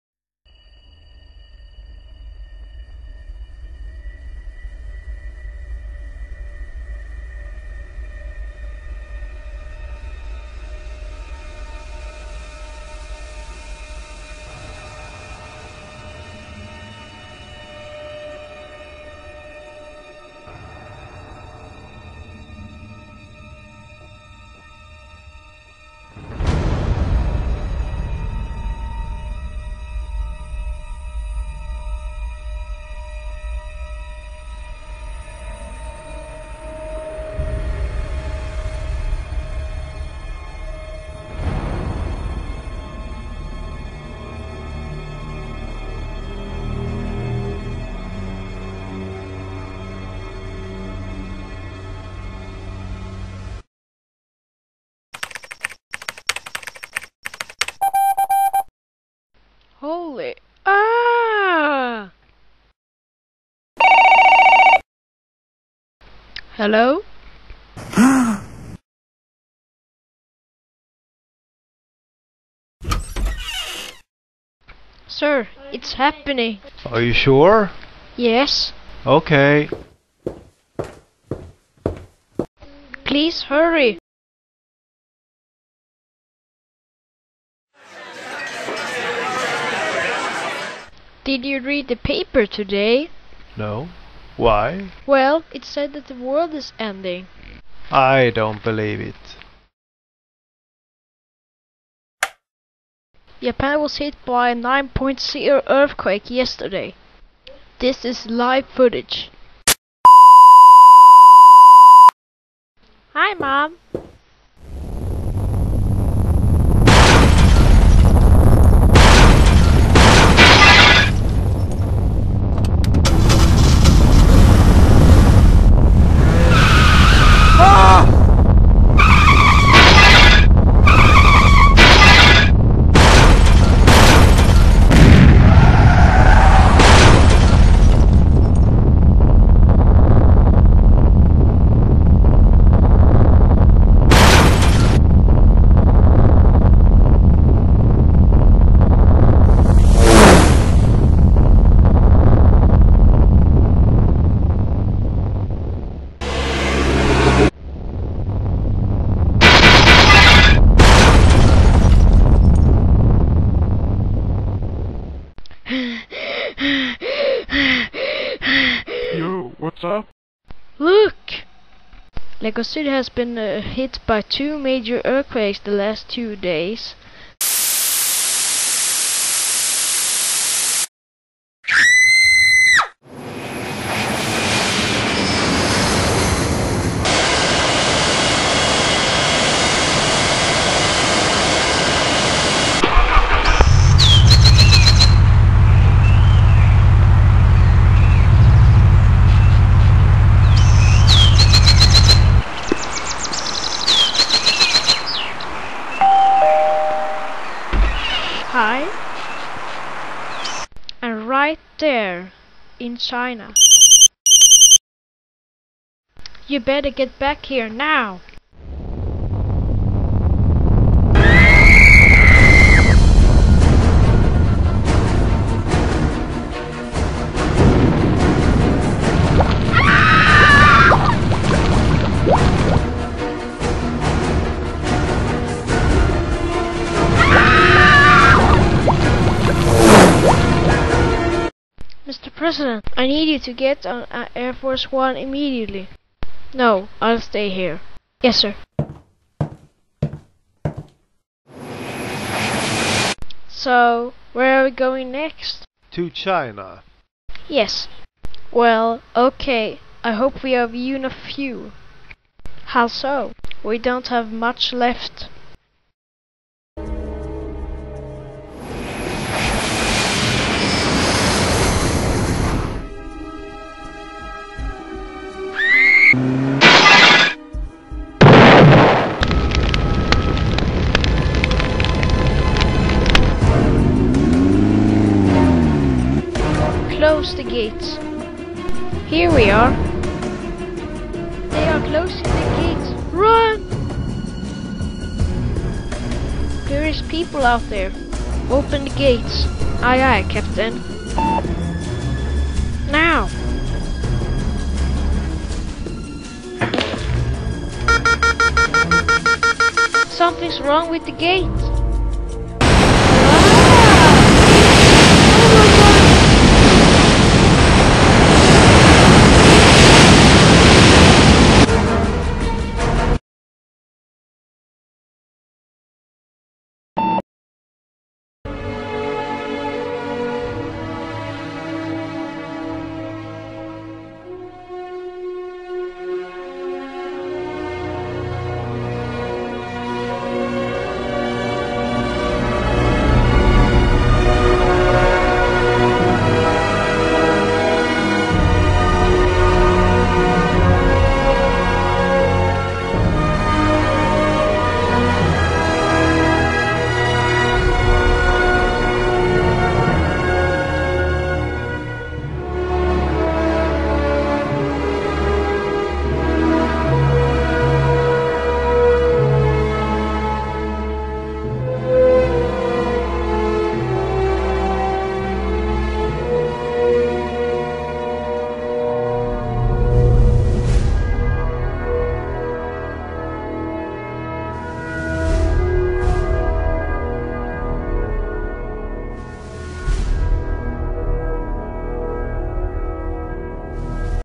Lego 2012
lego version of the movie 2012. A family in Los Angeles heads to China when the world is ending 'cause they heard of a group of ships in China those were made to survive the apocalypse.
Hope you like it, it took 8 weeks to finish! :P
NOTE: The sequence between 0:55 - 2:33 was shot on an Nikon Coolpix P2 and edited in Windows Movie Maker. The rest of the video was shot on a Logitech Webcam Pro 9000 and edited in Monkeyjam and Sony Vegas 9.0 Platinum Edition.

Honours: 2nd place in badgeramzinglegofilm's contest!
Nominated for best voice acting in EdcoStudios contest!